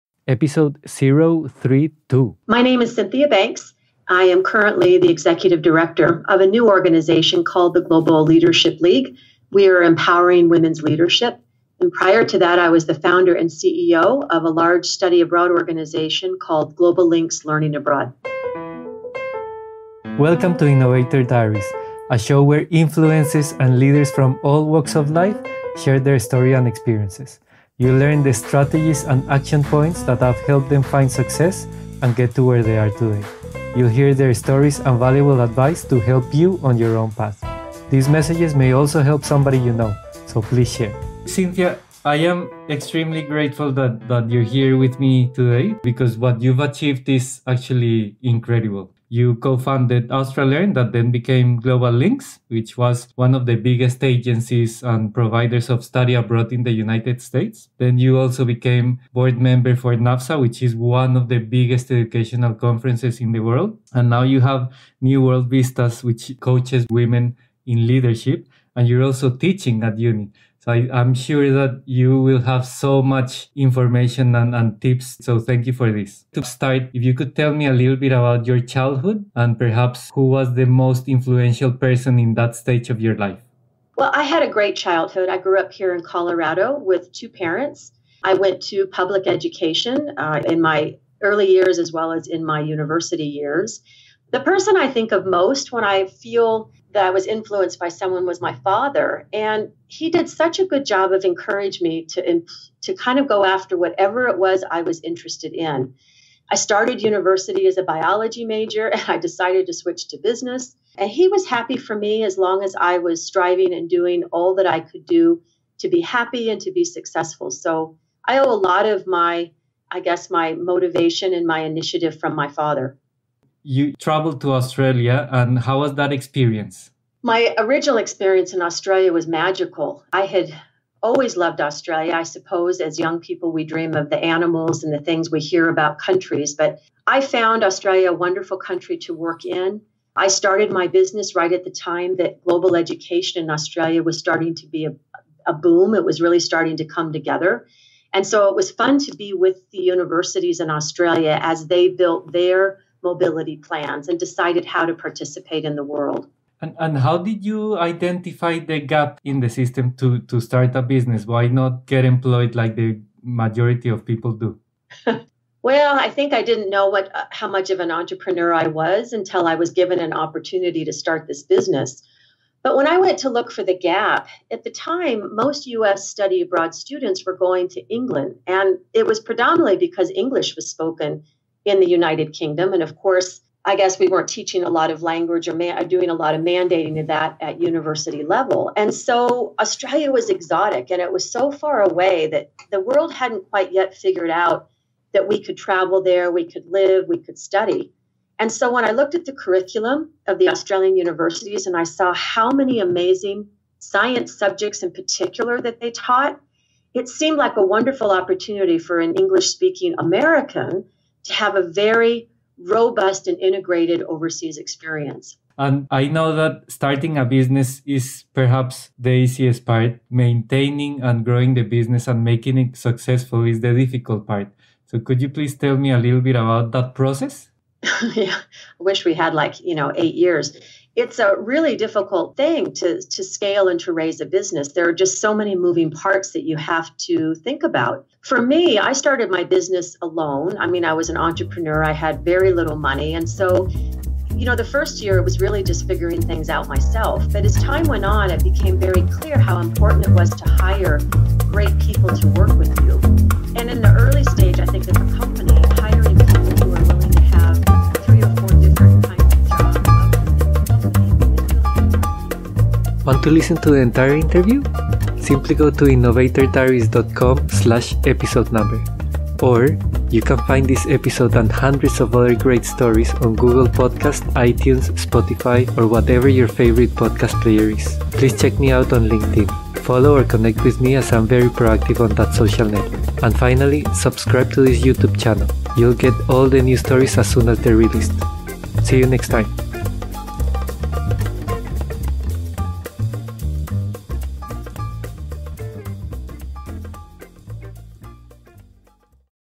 Global Education Specialist and Business owner, Cynthia banks
Cynthia banks is a top-level businesswoman. She is an influential name in the field of international education. She founded one of the most respected Study Abroad organisations in the world, is a global education specialist and an advocate for women’s Leadership. Her wealth of experience also placed her to be a board member for NAFSA – one of the biggest education conferences in the world. Cynthia banks  is the executive director of a new organisation called The Global Leadership League, which empowers women’s leadership. She is also the owner of consultancy business New World Vistas.